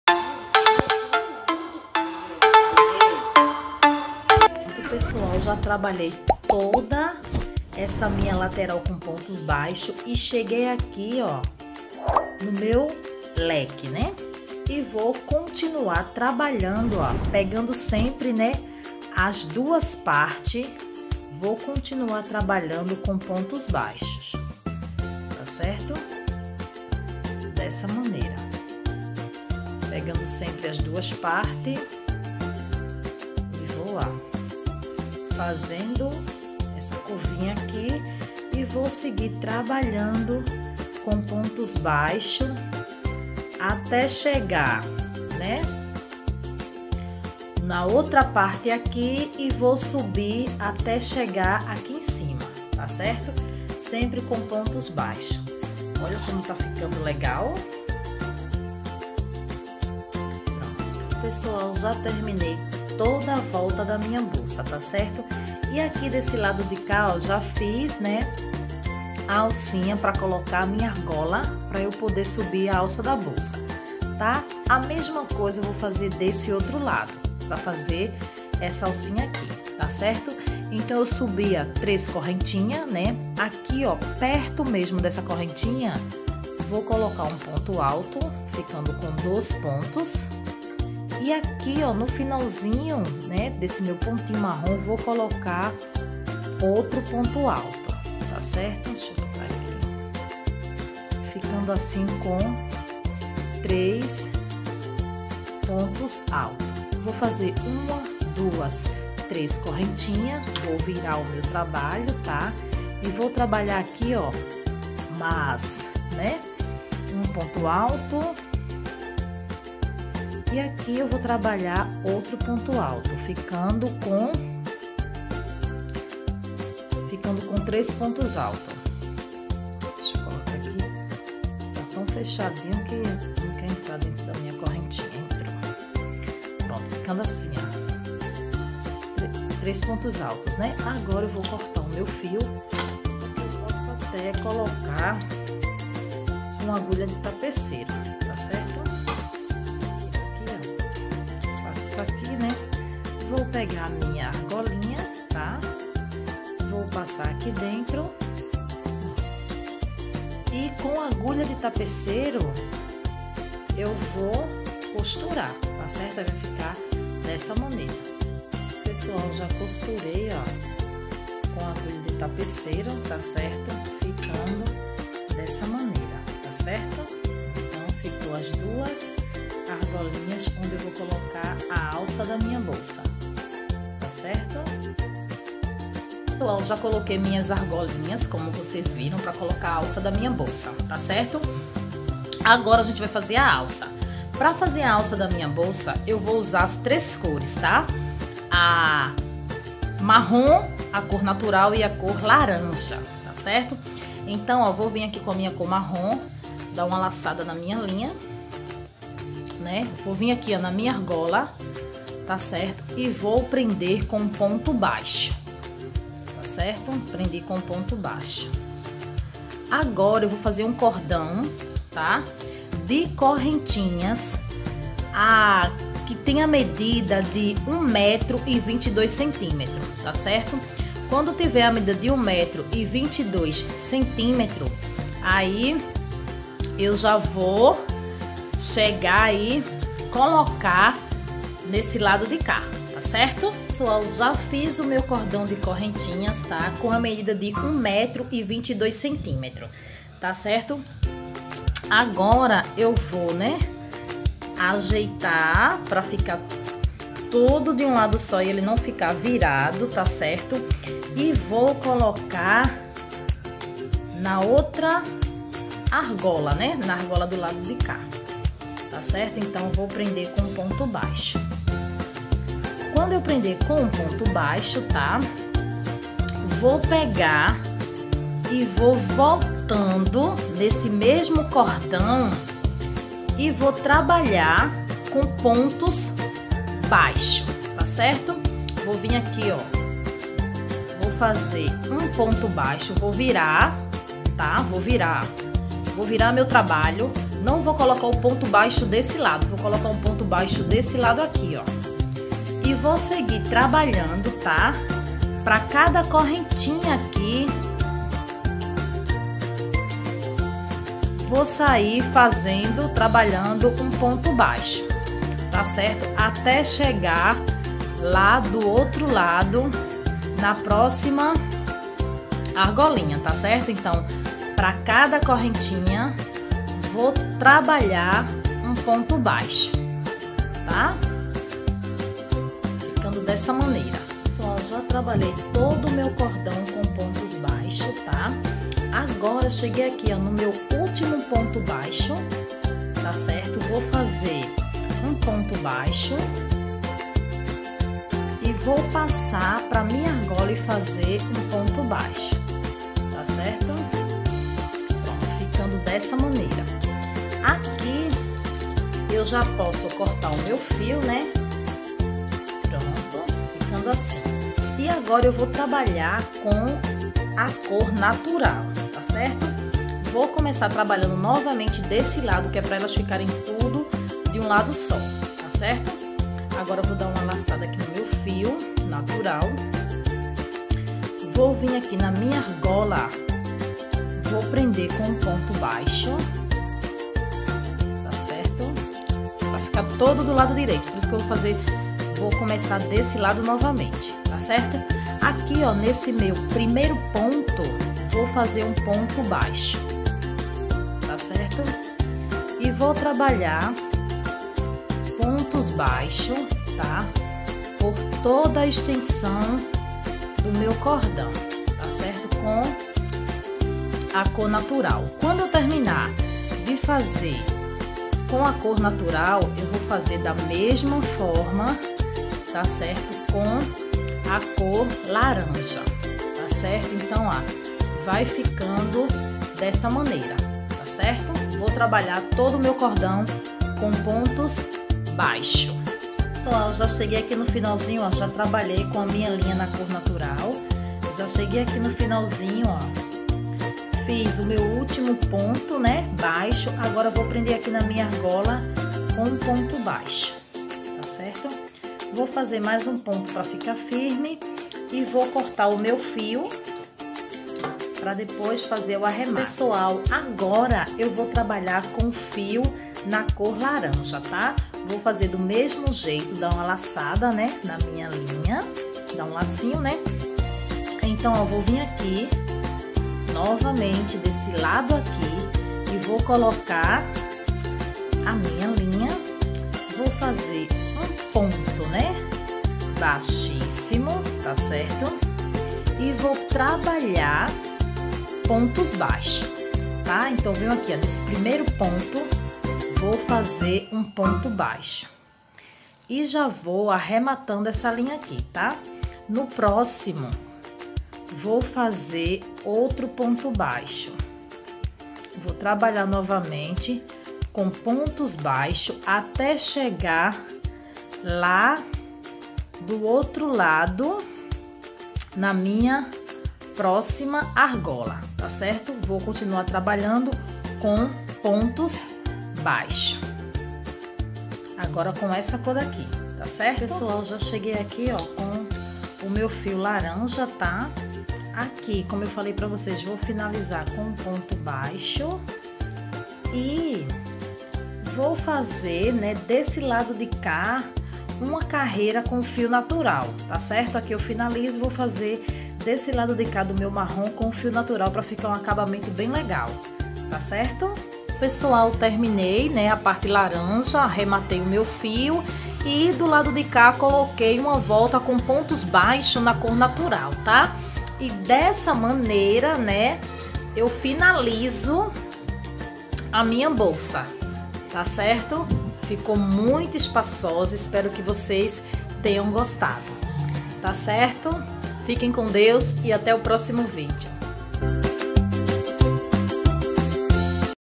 Bolsa Square em Crochê ateliêartessouza 2021 parte 5
Neste vídeo vou mostrar como fazer essa linda Bolsa em crochê.

Segue lá!